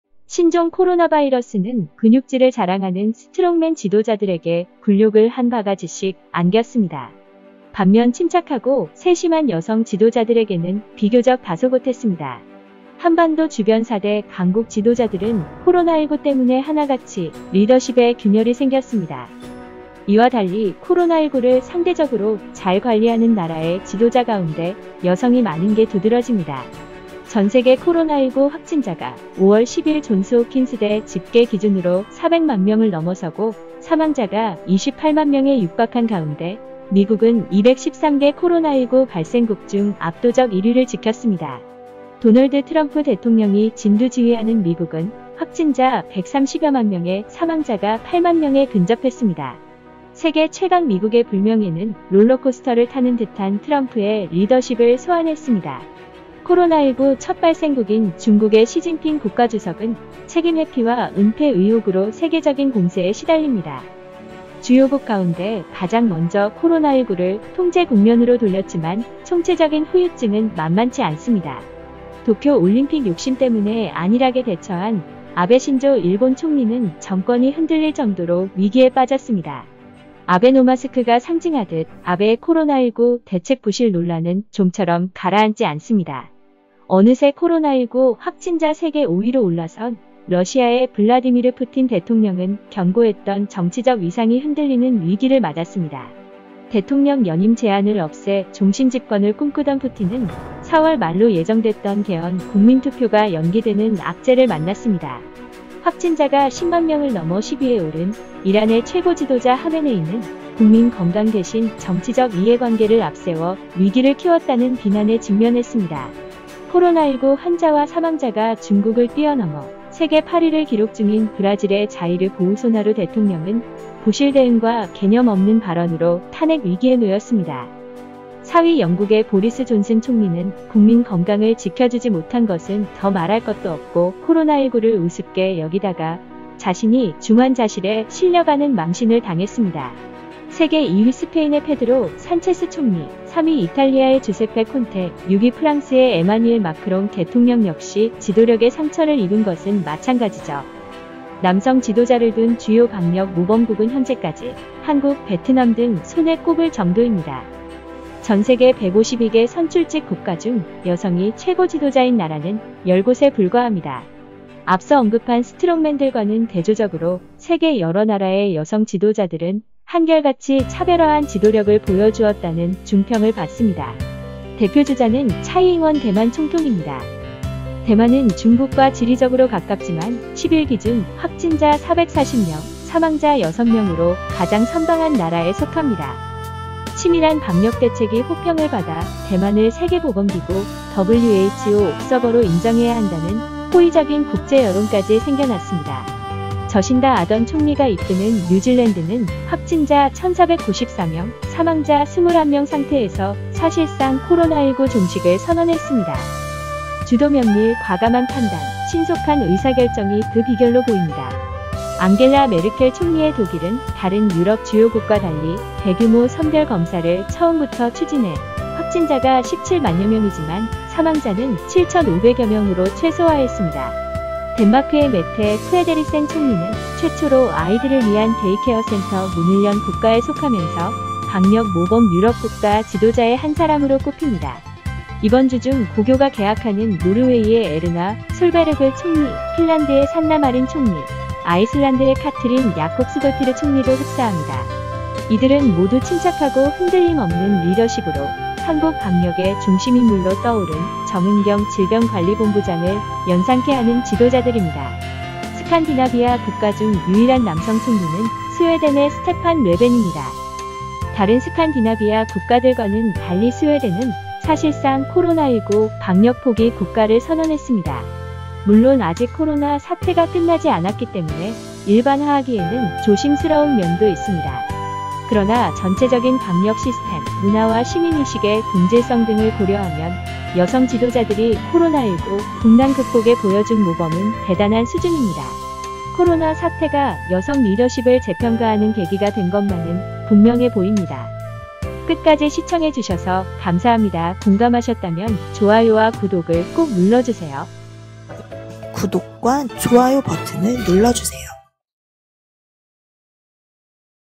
여성 지도자가 코로나19에 강하다? 트럼프·시진핑·아베·푸틴 스트롱맨 스타일 구겨 - 대만·뉴질랜드·독일·덴마크 등 모두 여성 지도자
신종 코로나바이러스는, 근육질을 자랑하는 스트롱맨 지도자들에게, 굴욕을 한 바가지씩 안겼습니다. 반면 침착하고 세심한 여성 지도자들에게는, 비교적 다소곳했습니다.
한반도 주변 4대 강국 지도자들은 코로나일구 때문에 하나같이 리더십에 균열이 생겼습니다. 이와 달리, 코로나일구를 상대적으로 잘 관리하는 나라의 지도자 가운데, 여성이 많은 게 두드러집니다.

전세계 코로나일구 확진자가, 5월 10일 존스홉킨스대 집계 기준으로 400만명을 넘어서고, 사망자가 28만명에 육박한 가운데, 미국은 213개 코로나일구 발생국 중 압도적 1위를 지켰습니다. 도널드 트럼프 대통령이 진두지휘하는 미국은, 확진자 130여만명에 사망자가 8만명에 근접했습니다. 세계 최강 미국의 불명예는, 롤러코스터를 타는 듯한 트럼프의 리더십을 소환했습니다. .

코로나일구 첫 발생국인 중국의 시진핑 국가주석은, 책임회피와 은폐 의혹으로 세계적인 공세에 시달립니다. 주요국 가운데 가장 먼저 코로나일구를 통제국면으로 돌렸지만, 총체적인 후유증은 만만치 않습니다.

도쿄 올림픽 욕심 때문에 안일하게 대처한 아베 신조 일본 총리는, 정권이 흔들릴 정도로 위기에 빠졌습니다. ‘아베노마스크’가 상징하듯, 아베의 코로나일구 대책 부실 논란은, 좀처럼 가라앉지 않습니다.

어느새 코로나일구 확진자 세계 5위로 올라선, 러시아의 블라디미르 푸틴 대통령은, 견고했던 정치적 위상이 흔들리는 위기를 맞았습니다. 대통령 연임 제한을 없애 종신집권을 꿈꾸던 푸틴은, 4월 말로 예정됐던 개헌 국민투표가 연기되는 악재를 만났습니다.

확진자가 10만명을 넘어 10위에 오른, 이란의 최고지도자 하메네이는, 국민 건강 대신 정치적 이해관계를 앞세워, 위기를 키웠다는 비난에 직면했습니다. 코로나일구 환자와 사망자가 중국을 뛰어넘어, 세계 8위를 기록 중인 브라질의 자이르 보우소나루 대통령은, 부실대응과 개념 없는 발언으로 탄핵 위기에 놓였습니다.

4위 영국의 보리스 존슨 총리는, 국민 건강을 지켜주지 못한 것은 더 말할 것도 없고, 코로나일구를 우습게 여기다가 자신이 중환자실에 실려가는 망신을 당했습니다.

세계 2위 스페인의 페드로 산체스 총리, 3위 이탈리아의 주세페 콘테, 6위 프랑스의 에마뉘엘 마크롱 대통령 역시, 지도력에 상처를 입은 것은 마찬가지죠. 남성 지도자를 둔 주요 방역 모범국은 현재까지 한국 베트남 등 손에 꼽을 정도입니다.

전세계 152개 선출직 국가 중 여성이 최고지도자인 나라는 10곳에 불과합니다. 앞서 언급한 스트롱맨들과는 대조적으로, 세계 여러 나라의 여성 지도자들은, 한결같이 차별화한 지도력을 보여주었다는 중평을 받습니다.
대표주자는 차이잉원 대만 총통입니다. 대만은 중국과 지리적으로 가깝지만, 확진자 440명, 사망자 6명(10일 기준)으로 가장 선방한 나라에 속합니다. 치밀한 방역대책이 호평을 받아, 대만을 세계보건기구(WHO) 옵서버로 인정해야 한다는, 호의적인 국제여론까지 생겨났습니다.

저신다 아던 총리가 이끄는 뉴질랜드는, 확진자 1494명, 사망자 21명 상태에서 사실상 코로나일구 종식을 선언했습니다. 주도면밀, 과감한 판단, 신속한 의사결정이 그 비결로 보입니다.

앙겔라 메르켈 총리의 독일은, 다른 유럽 주요국과 달리, 대규모 선별 검사를 처음부터 추진해, 확진자가 17만여명이지만, 사망자는 7500여명으로 최소화했습니다.

덴마크의 메테 프레데릭센 총리는, 최초로 아이들을 위한 데이케어센터 문을 연 국가에 속하면서, 방역 모범 유럽국가 지도자의 한 사람으로 꼽힙니다.

이번주 중·고교가 개학하는 노르웨이의 에르나 솔베르그 총리, 핀란드의 산나 마린 총리, 아이슬란드의 카트린 야콥스도티르 총리도 흡사합니다. 이들은 모두 침착하고 흔들림 없는 리더십으로 한국 방역의 중심인물로 떠오른, 정은경 질병관리본부장을 연상케 하는 지도자들입니다.

스칸디나비아 국가 중 유일한 남성 총리는, 스웨덴의 스테판 뢰벤입니다. 다른 스칸디나비아 국가들과는 달리 스웨덴은 사실상 코로나일구 방역 포기 국가를 선언했습니다.

물론 아직 코로나 사태가 끝나지 않았기 때문에, 일반화하기에는 조심스러운 면도 있습니다. 그러나 전체적인 방역 시스템, 문화와 시민의식의 동질성 등을 고려하면, 여성지도자들이 코로나일구 국난 극복에 보여준 모범은 대단한 수준입니다. 코로나 사태가 여성 리더십을 재평가하는 계기가 된 것만은 분명해 보입니다.